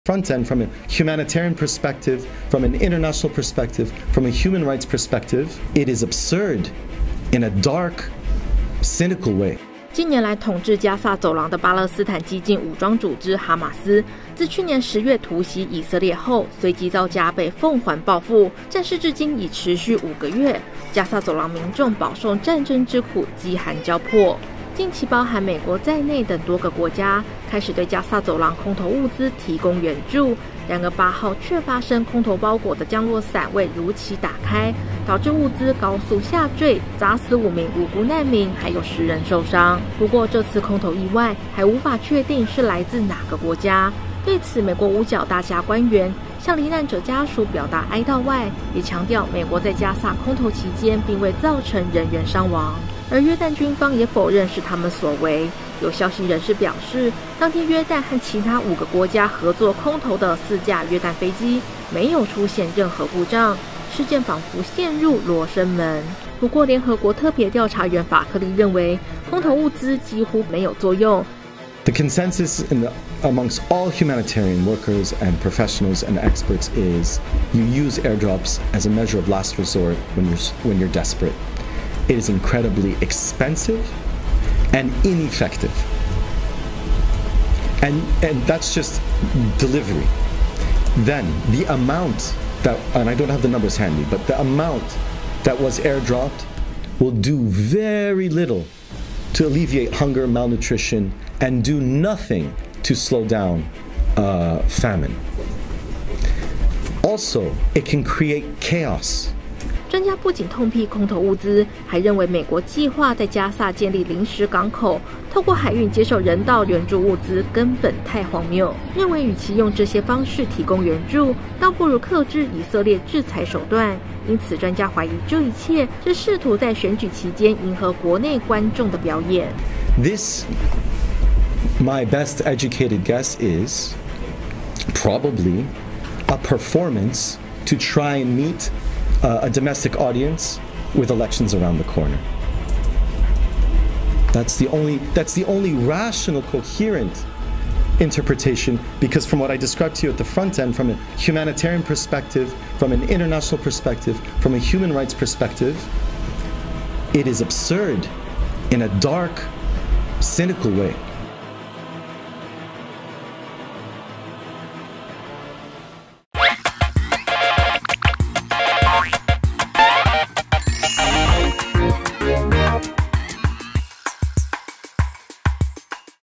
加薩「空投物資」砸死人！ 降落傘打不開失速墜落釀死傷
影片授權：Xinhua News Agency、DVIDS、CCTV／CGTN、路透社